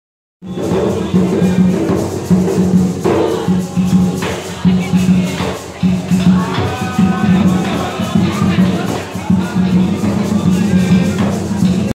Restauración Cultural taller de bomba Girls Scout
Taller de bomba realizado por Yadilka Rodriguez,  Felipe Febres y Pablo Luis Rivera de Restauración Cultural.  Sumando! !+